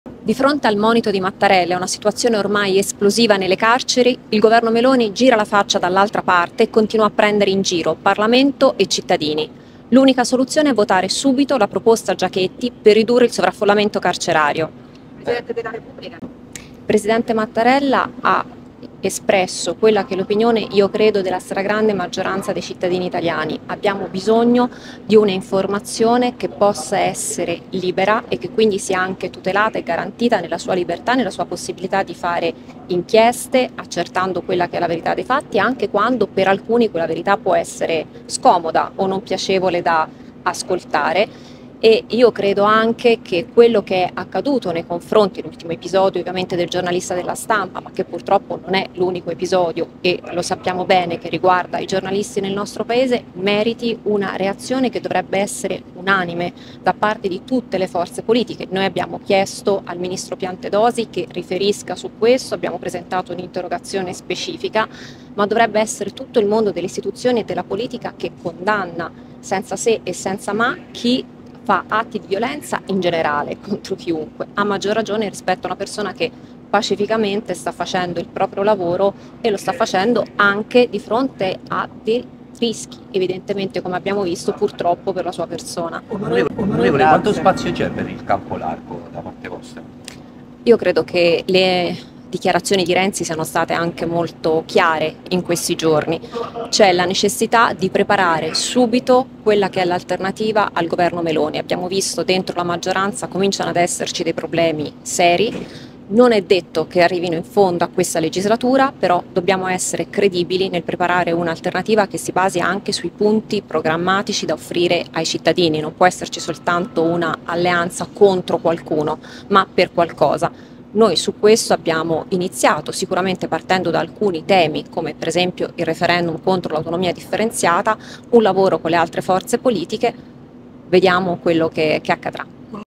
Dl Carceri, Boschi: “Meloni prende in giro i cittadini, serve unità per contrastare il governo”
Dl Carce, Boschi: “Meloni prende in giro i cittadini, serve unità per contrastare il governo”

Video di Michele Lilla